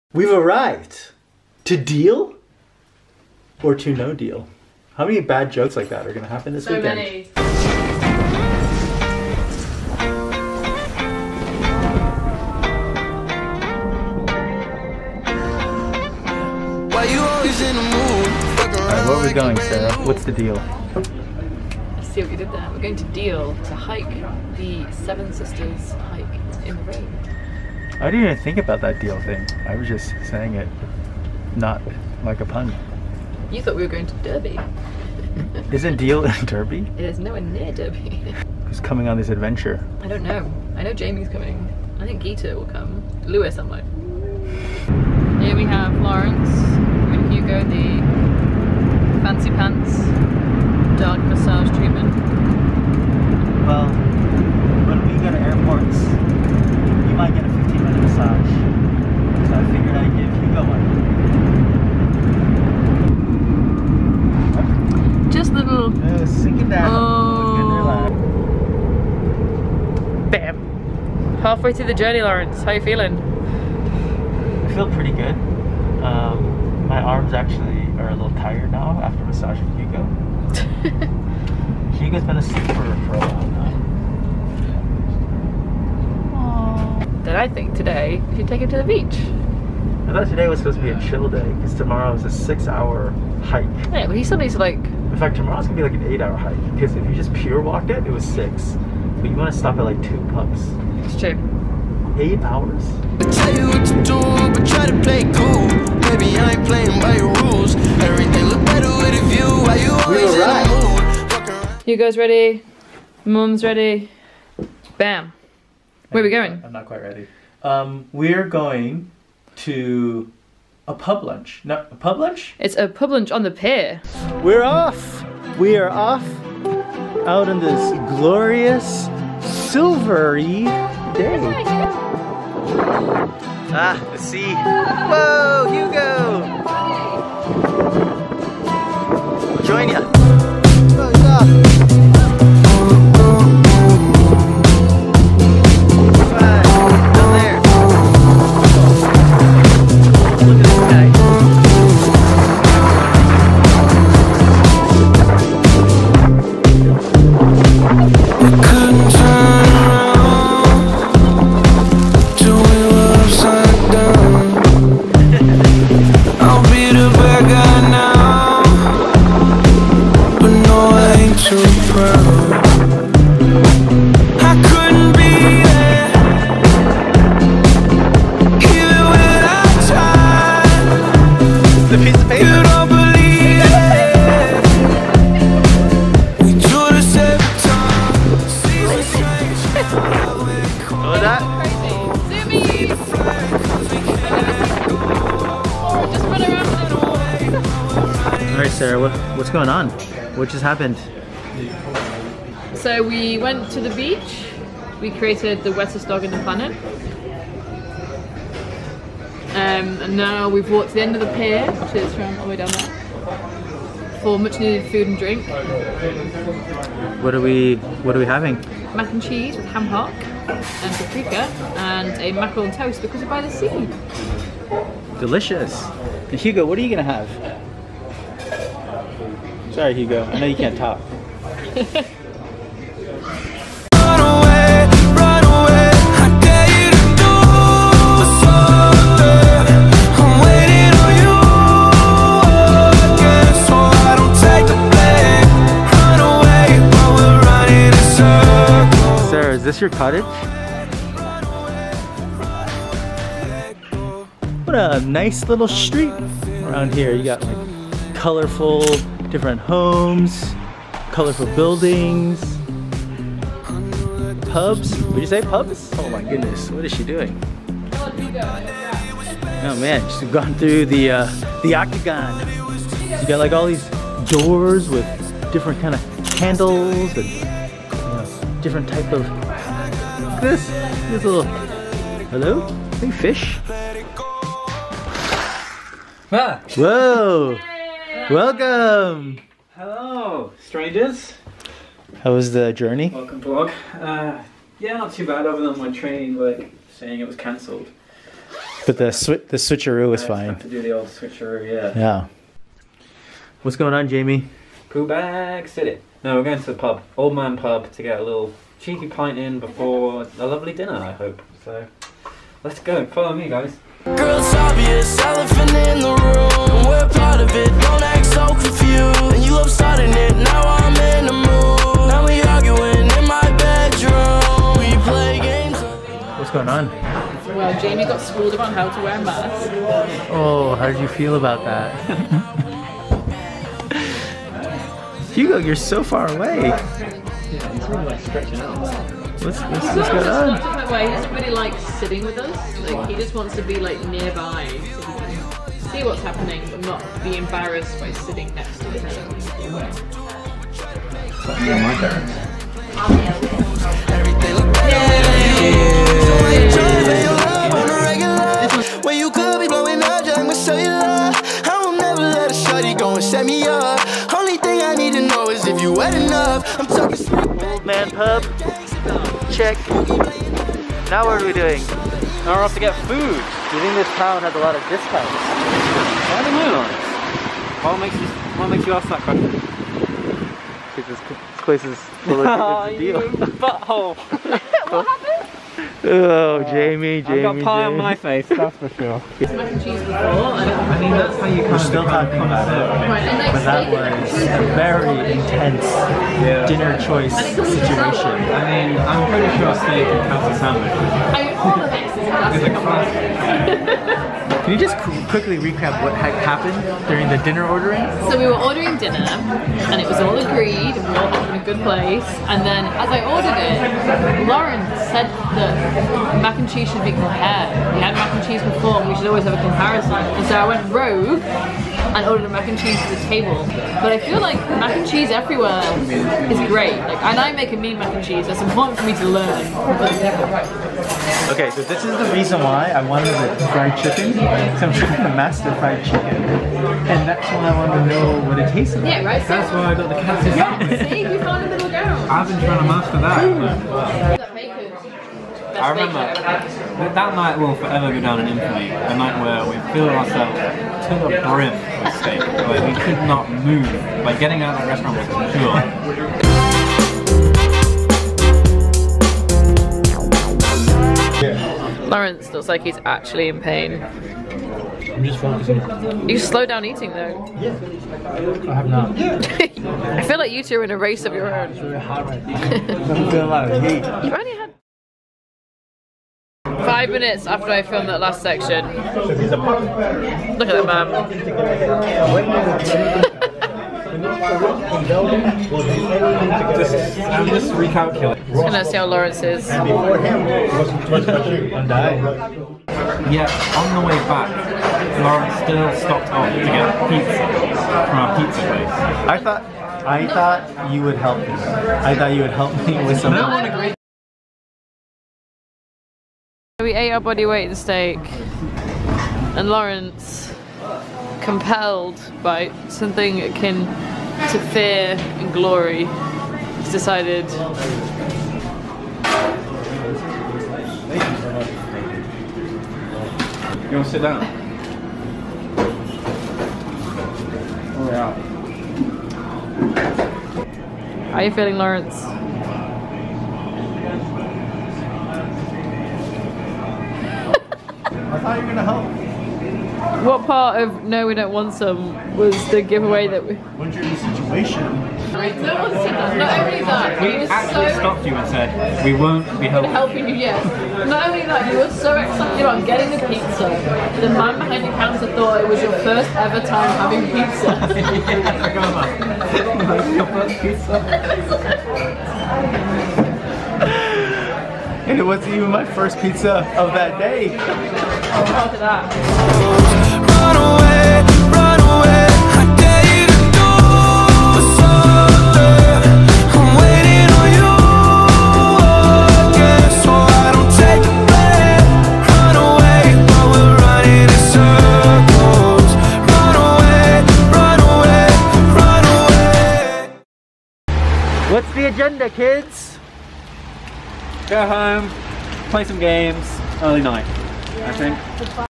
Hugo's First Trip Away | White Cliffs of Dover | Day 1
Since the world has been in lockdown, we haven't thought about traveling or going on holiday at all. Just over 3 months ago, we decided to adopt a Romanian rescue dog, Hugo, and he's forever changed our lives. For his 3-month anniversary, we decided to take him on his first trip (by train, which he's way more comfortable on) to the coastal town of Deal.

On this trip our good friends Jamie and Geetha joined us, following all safety rules of course, for a good ole fashion hike and photo mission of the White Cliffs of Dover! But Day 1 was just lots of delicious food and a run on a very wet beach.